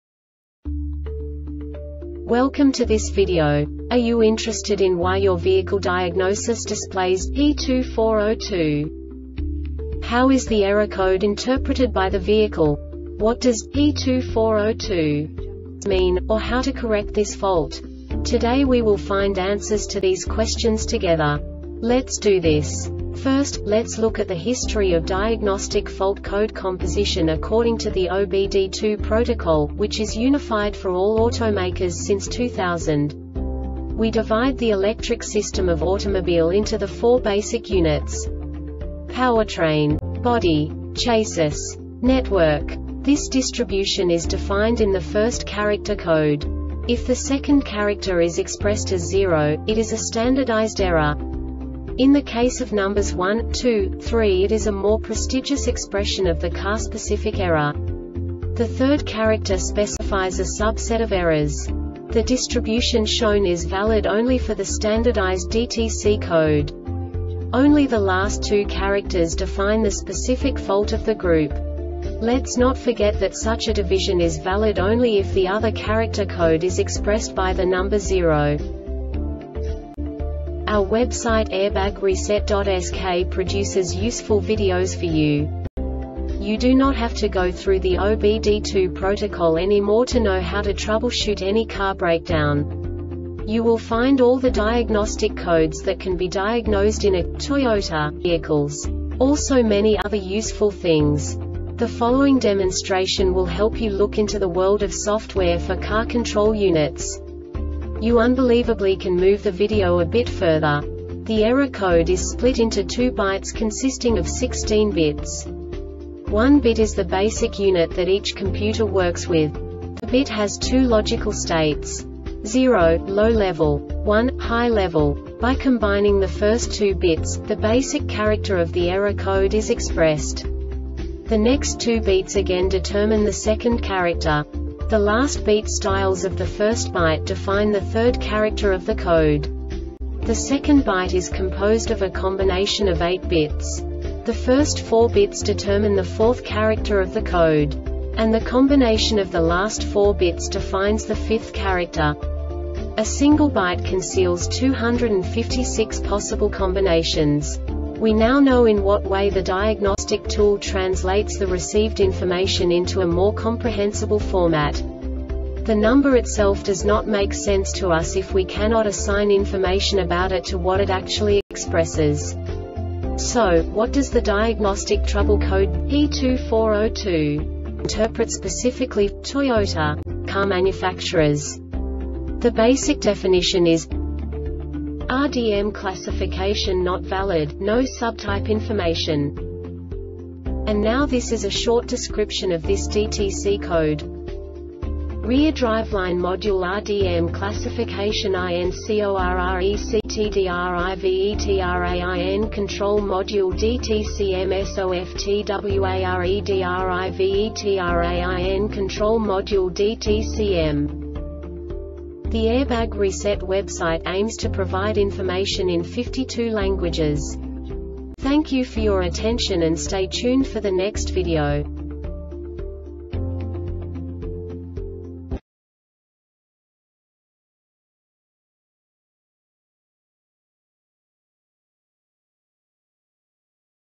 DTC Toyota P2402 Short Explanation
Contents:
0:21 Basic DTC analysis according to OBD2 protocol standard.
1:48 Insight into programming
2:58 A diagnostically understandable description of the Diagnostic Trouble Code
3:05 Basic error definition

Make:
Toyota
Code:
P2402
Definition:
RDM Classification Not Valid - No Subtype Information
Cause:
REAR DRIVELINE MODULE (RDM) CLASSIFICATION INCORRECTDRIVETRAIN CONTROL MODULE (DTCM) SOFTWAREDRIVETRAIN CONTROL MODULE (DTCM)